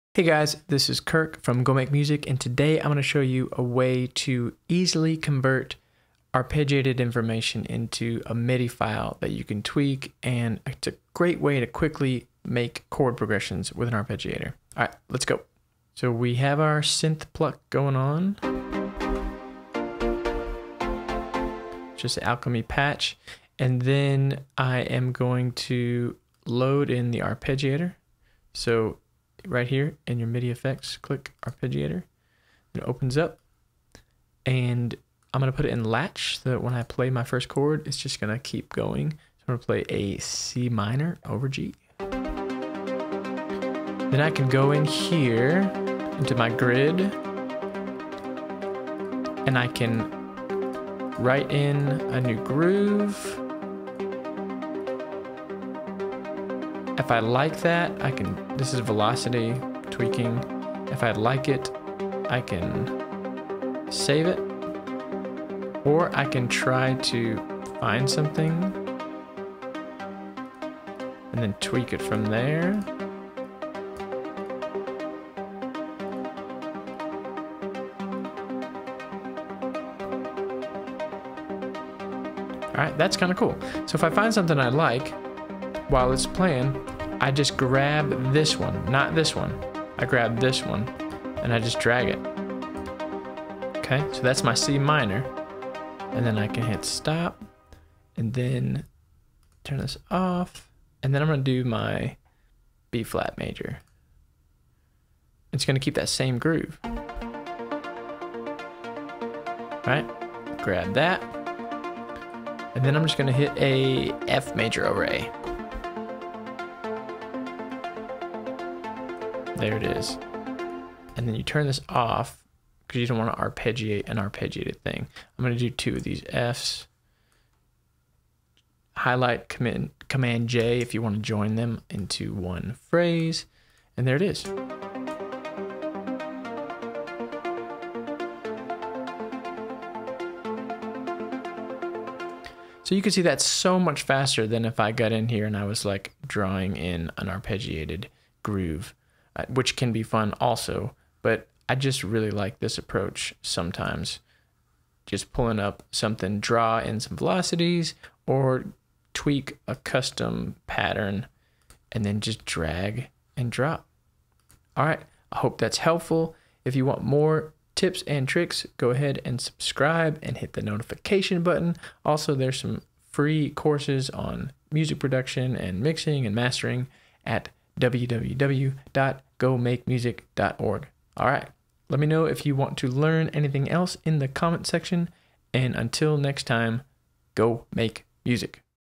Logic Pro X Arpeggiator to MIDI Hack #1
logic pro x arpeggiator to midi is great for quickly making chord progressions and having flexible tweak-able arpeggios.